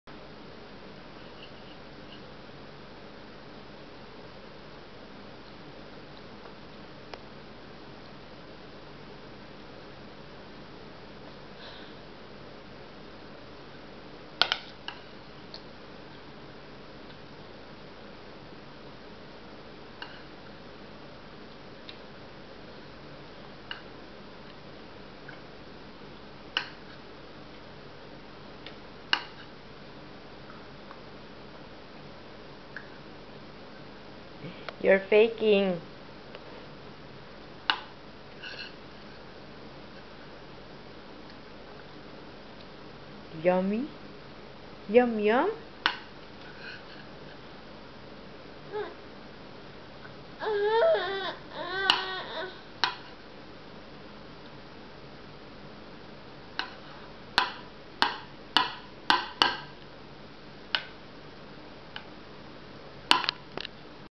Eating Adobo
These are one of the less than stressful meal times. My baby likes adobo and he wants to eat without help